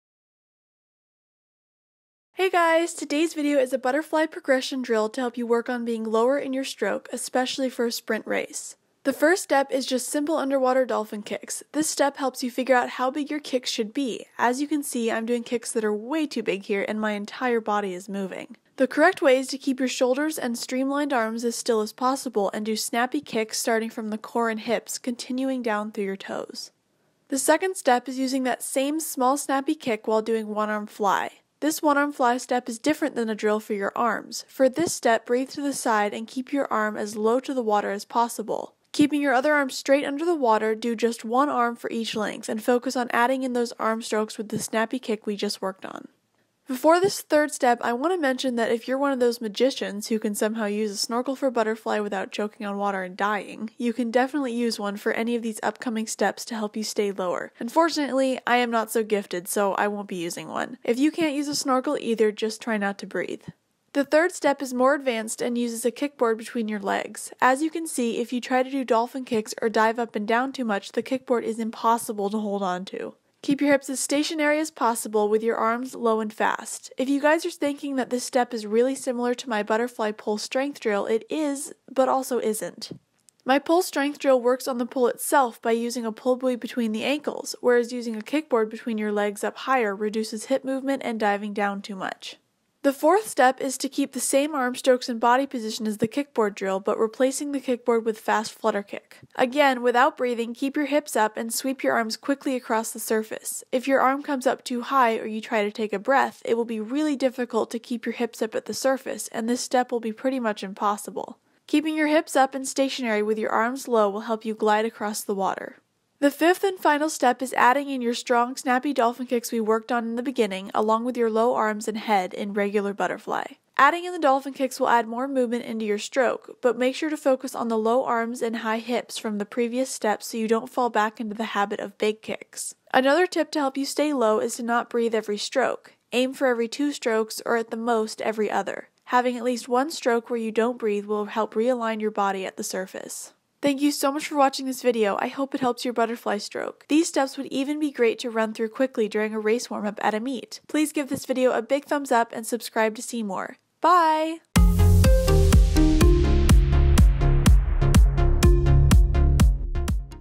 Progression Steps for LOW Fly! Butterfly Sprint Drill!
Today's video are 5 progression steps for LOW butterfly! :)

Speedo Performance Bullet Head Snorkel
On Amazon

GoPro Hero5
On Amazon

~Lindsey